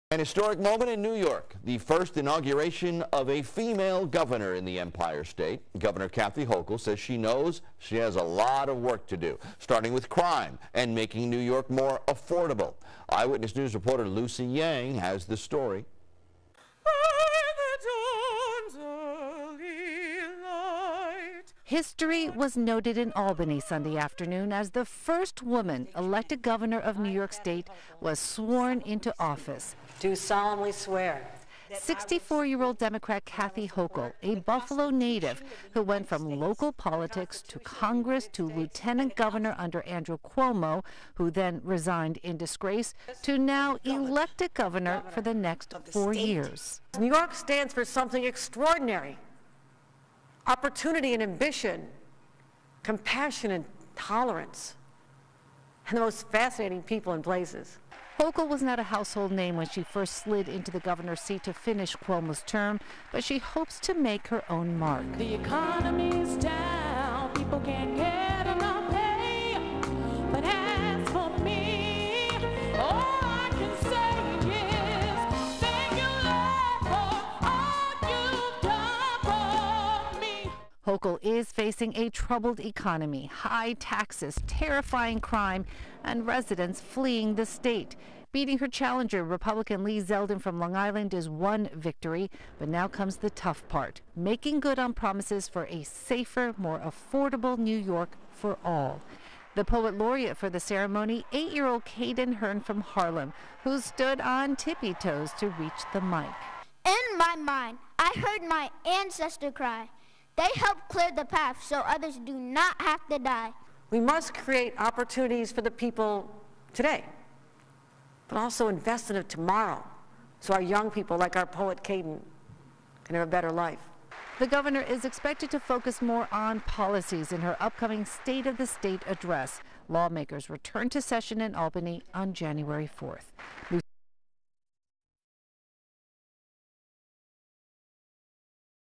Hochul becomes 1st female governor inaugurated in New York
New York Gov. Kathy Hochul was sworn in Sunday for her first full term in office and deliver her inaugural address. Lucy Yang has more.